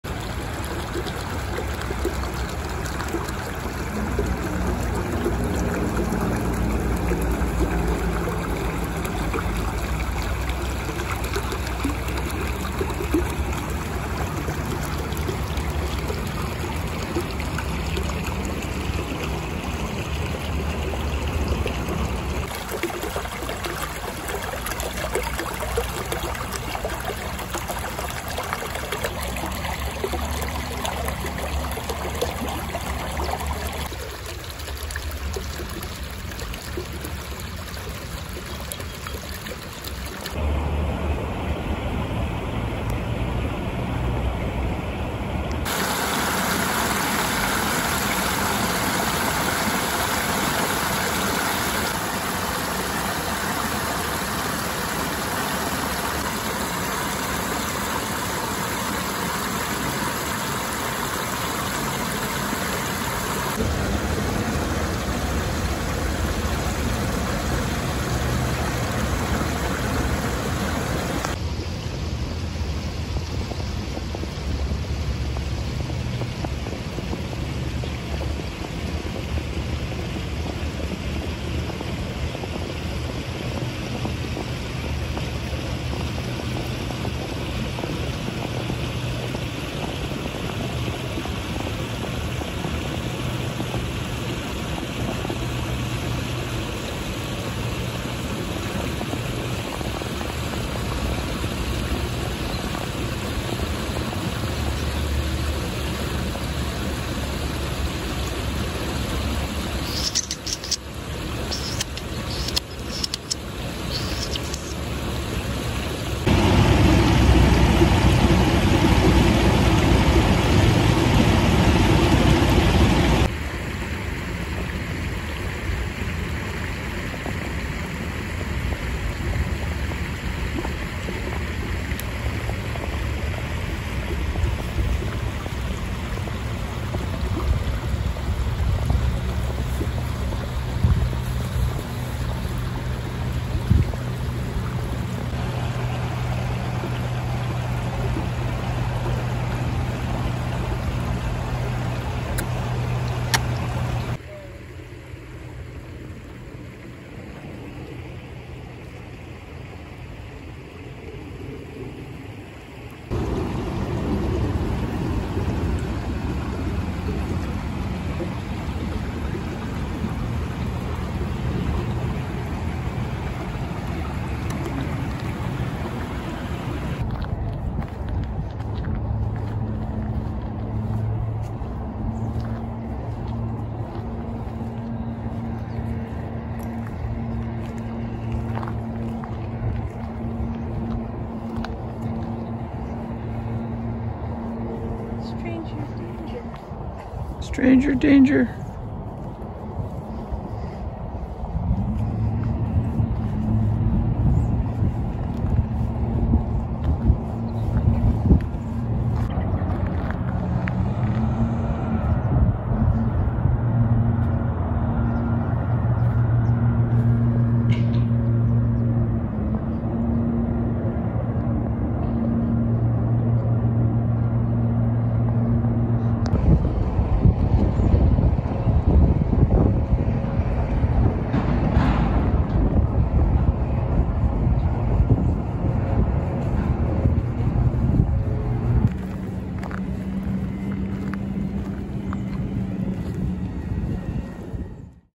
Meg Walk Creek
Went for a nice full sun walk with Meg near the local creek! Enjoy the wildlife, birds hunting insects!